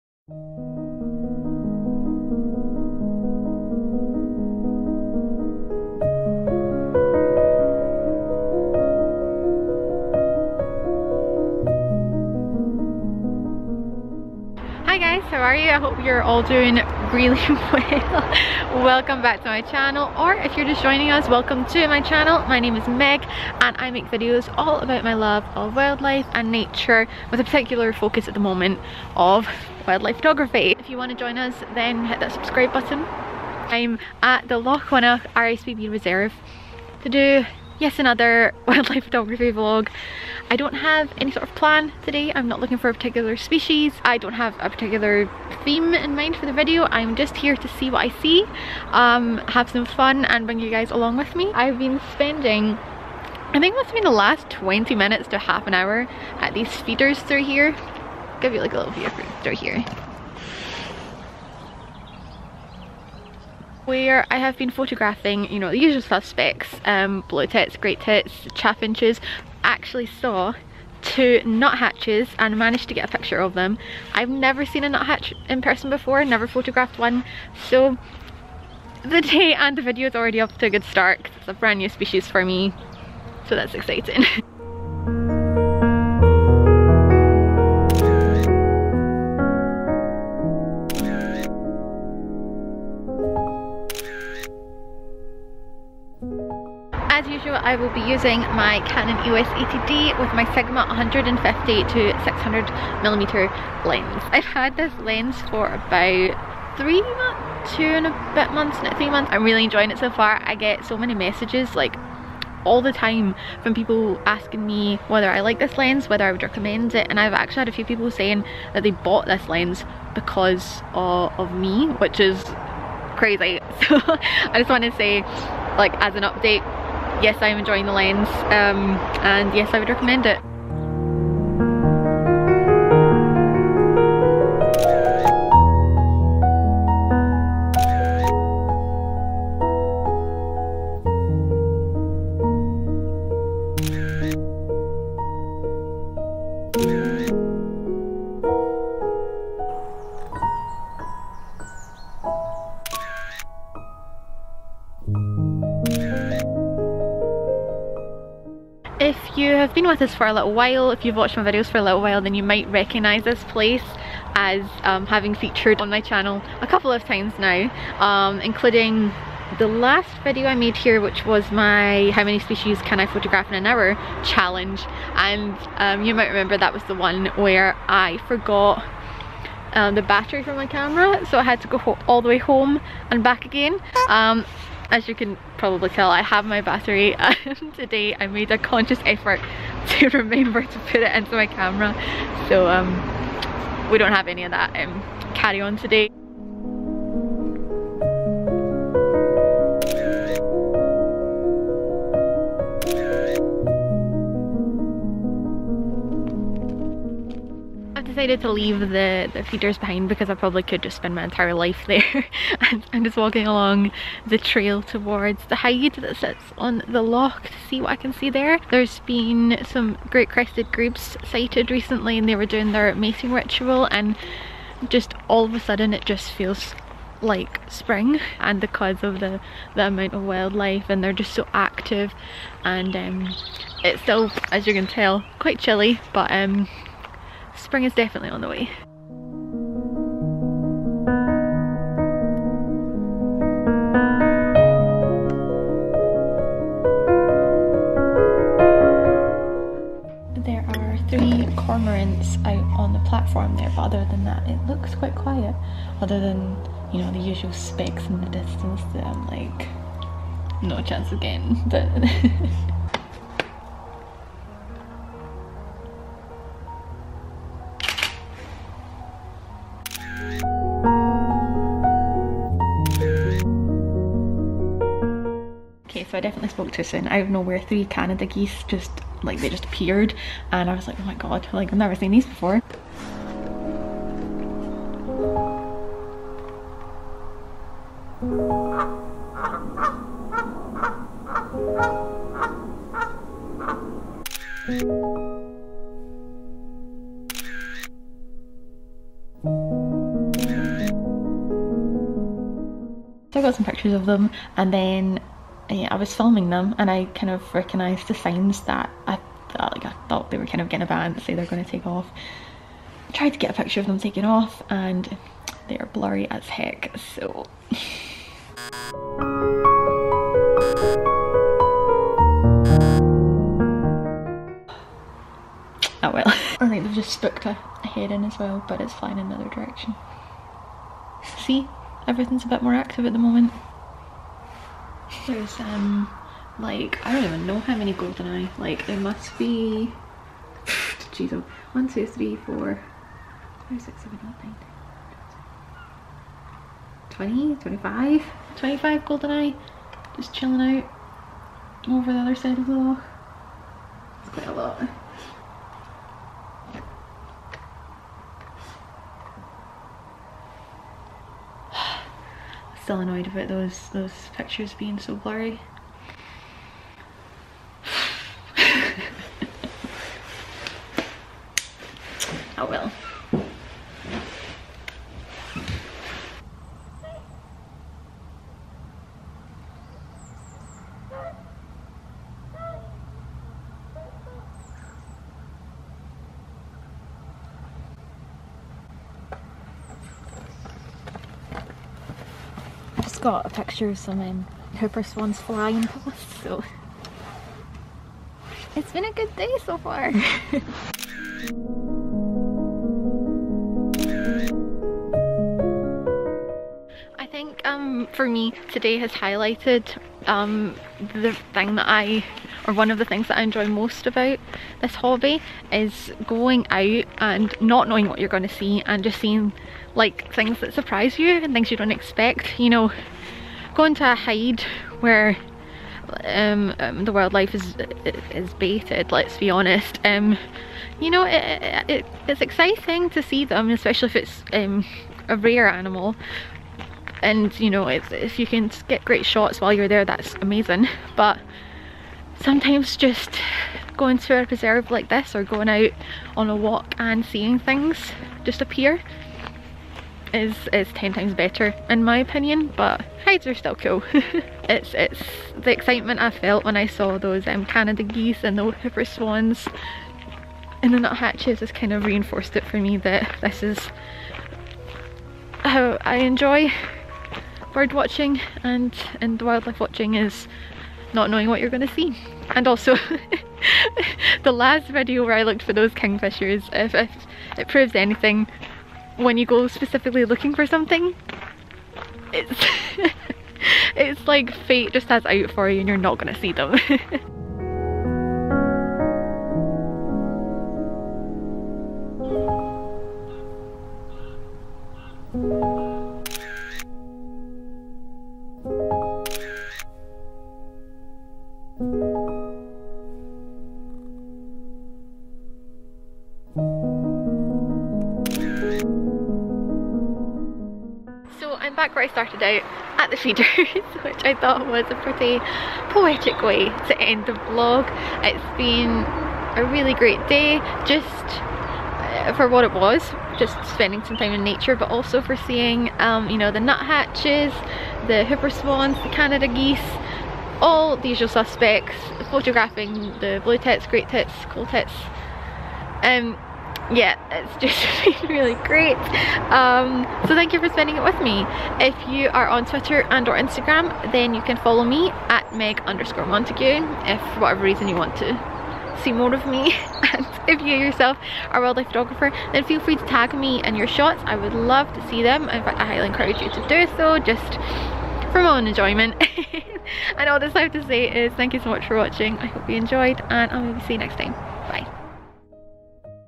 Photographing 3 new species! Nuthatch, Canada Geese and Whooper Swan | Wildlife Photography Vlog #17
I went for a walk at the RSPB's Lochwinnoch Nature Reserve and saw 3 species I'd never seen before!!

Come say hi!

Wildlife spotted:
Nuthatch (Sitta europaea)
Chaffinch (Fringilla coelebs)
Blackbird (Turdus merula)
Coal tit (Periparus ater)
Blue tit (Cyanistes caeruleus)
Great tit (Parus major)
Robin (Erithacus rubecula)
Canada goose (Branta canadensis)
Whooper swan (Cygnus cygnus)
Moorhen (Gallinula chloropus)
Cormorant (Phalacrocorax carbo)

My equipment:
Vlogging camera: Canon G7X Mark II
Main camera: Canon EOS 80D
Lens: Sigma 150-600mm Contemporary Lens (Canon Fit)

Meg xx